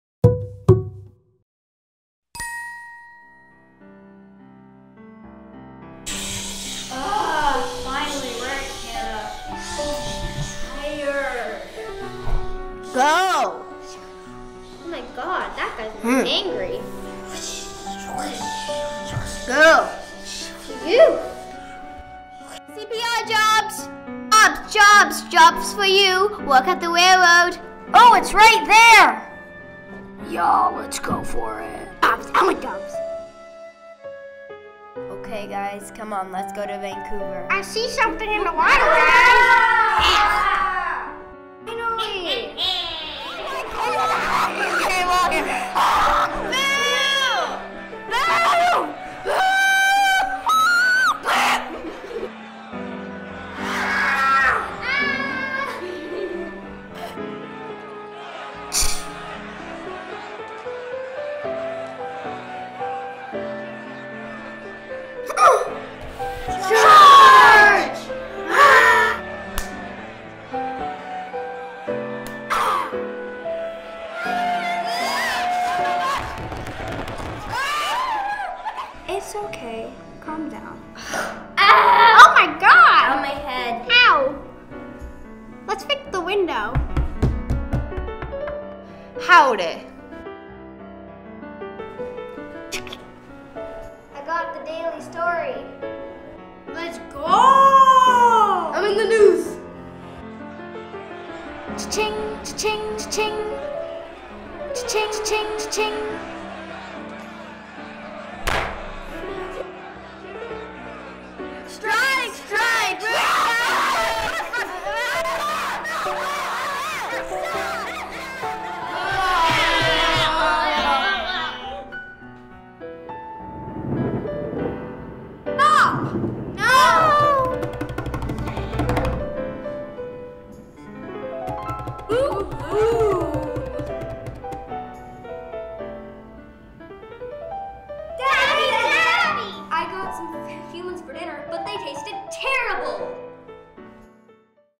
Anti-Asian Riots of 1907 - Lord Tennyson Elementary School (February 27, 2024)
This short was done at the Museum of Vancouver on February 27, 2024 in a one-day workshop called Animating History. A short animation demonstration taught them how to do cut out animation. Sound was recorded separately after groups had completed their second scene. Animating History is a partnership of the Museum of Vancouver and the Reel 2 Real International Film Festival for Youth.

The 1907 Vancouver riots, occurring over September 7–9, were part of a wave of anti-Asian violence on the West Coast of North America. Triggered by widespread anti-immigrant sentiment, they reflected tensions towards Asian immigrants, particularly Chinese, Japanese, and South Asians. Labour unions and small businesses fueled agitation for direct action. While no deaths occurred, extensive damage was inflicted on Asian-owned property. The influx of Asian migrants, economic downturns like the Panic of 1907, and perceived job competition exacerbated racial tensions. The riots were a culmination of growing anti-Asian sentiment, leading to mob violence and destruction of property in Chinatown and Japantown. The aftermath included government compensation for damages and implementation of immigration bans, notably the "gentlemen's agreement" and continuous journey regulation targeting Asian immigrants. These events also reverberated internationally, influencing discussions on immigration policies and alliances.